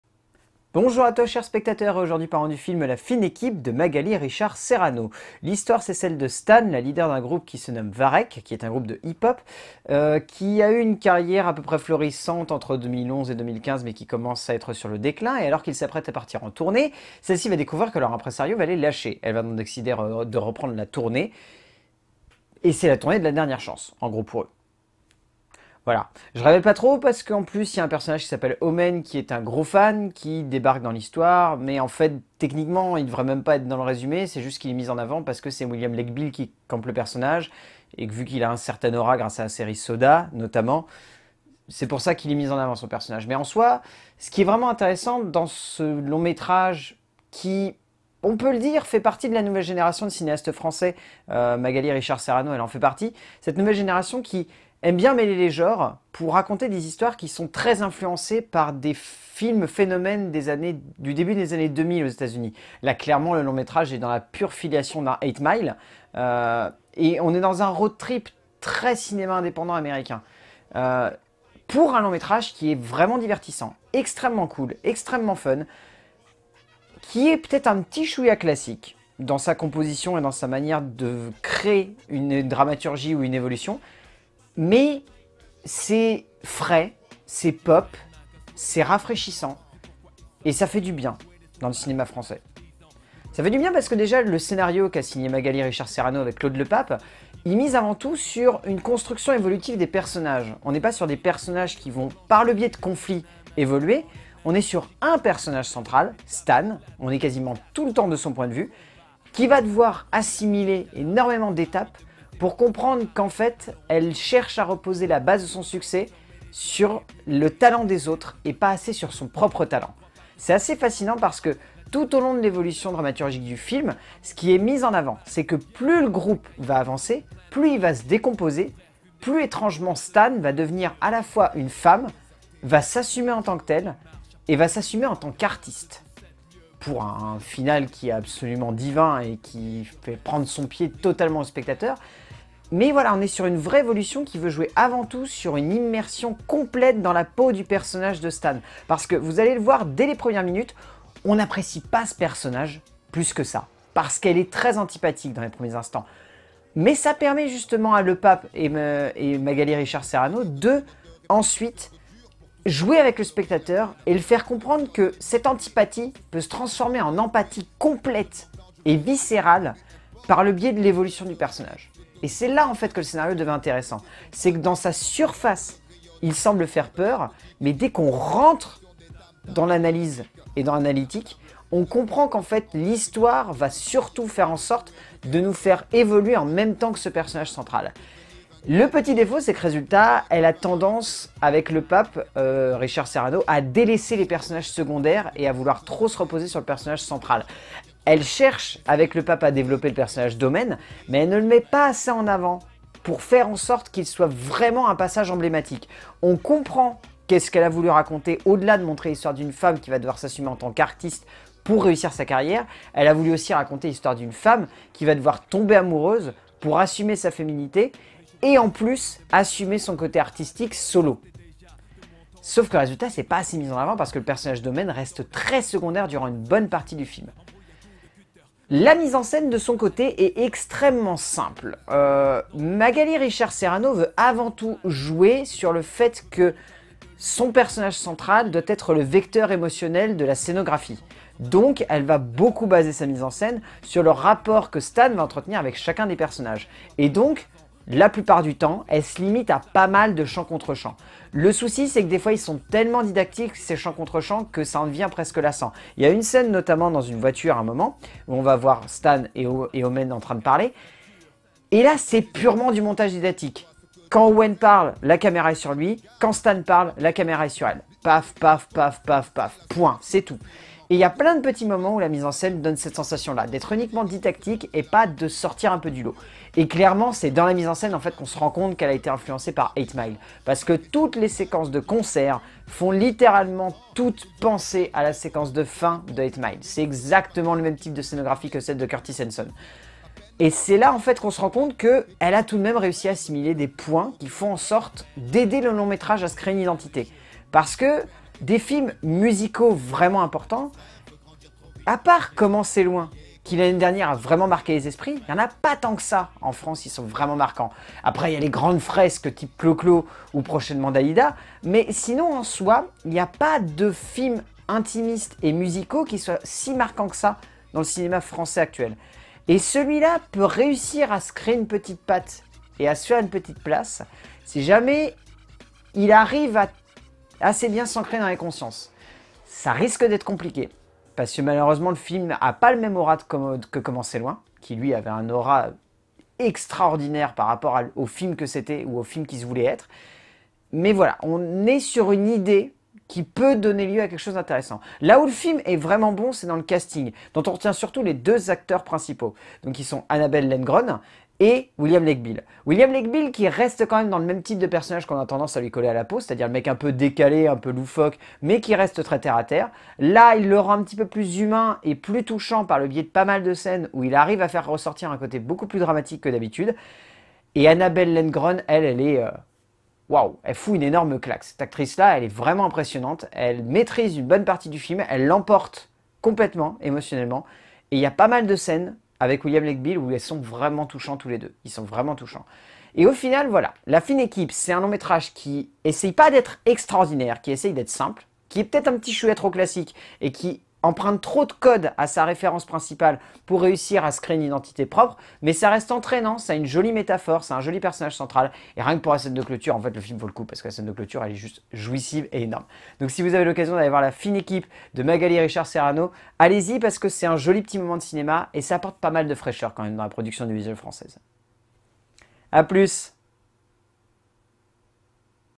LCDC - La fine équipe
Vlog critique sur la sortie d'une comédie en forme de road trip particulièrement fun, et décalée, pour un vrai plaisir de cinéma moderne et classe, "La fine équipe"